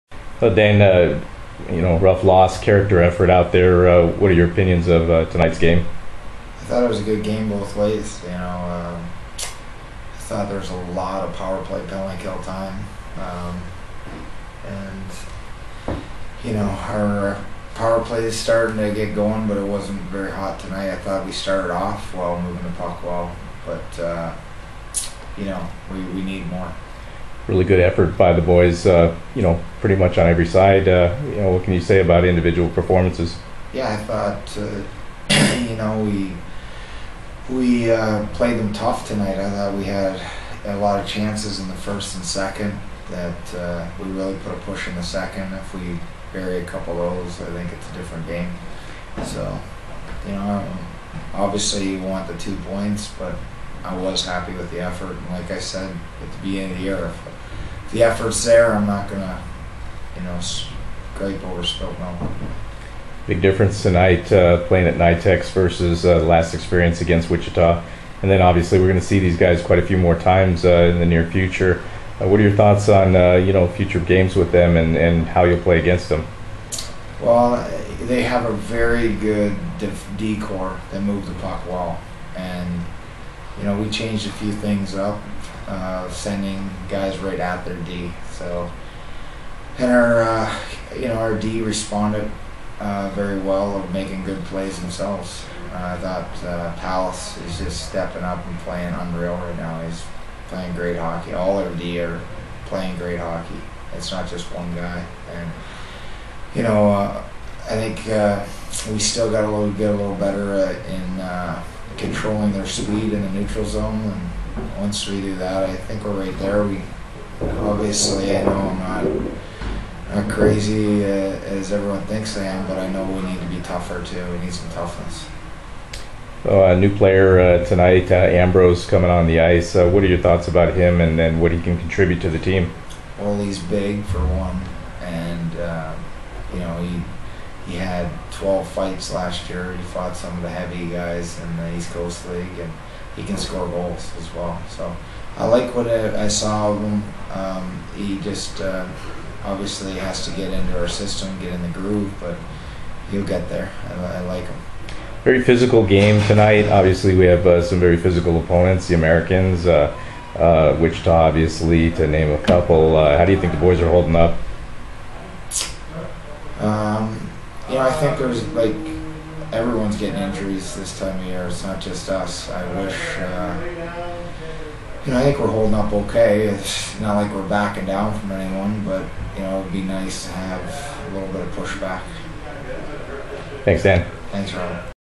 Fort Worth Brahmas Post-Game Coach's Wrap - 12/16/12
Fort Worth Brahmas Head Coach Dan Wildfong talks about the team's 4-3 shootout loss to the Wichita Thunder